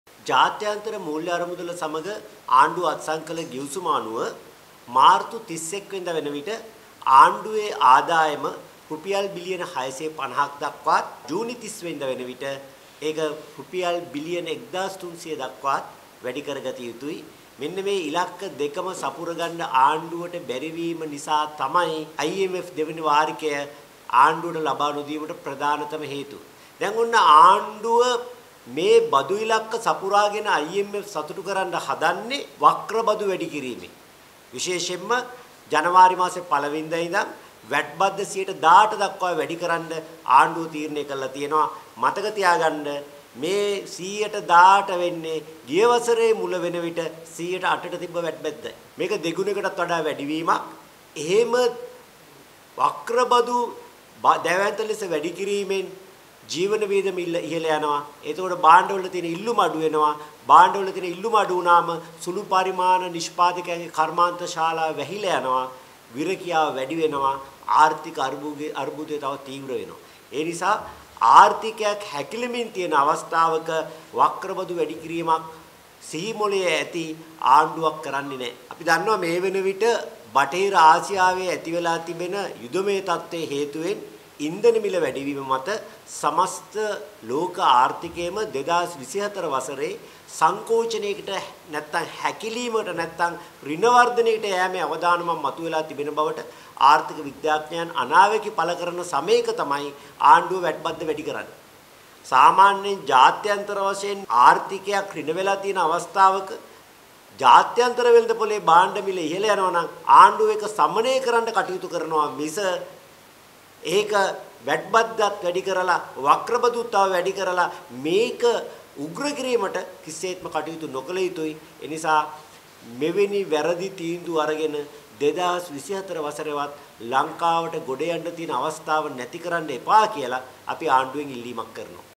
වැට් 8% සිට 18% දක්වා නගී, ආර්ථිකය හැකිලීමේ ආණ්ඩුවේ කුමන්ත්‍රණය මෙන්න
වැට් 8% සිට 18% දක්වා නගී,
ආර්ථිකය හැකිලීමේ ආණ්ඩුවේ කුමන්ත්‍රණය මෙන්න

පිවිතුරු හෙළ උරුමය මාධ්‍ය හමුව - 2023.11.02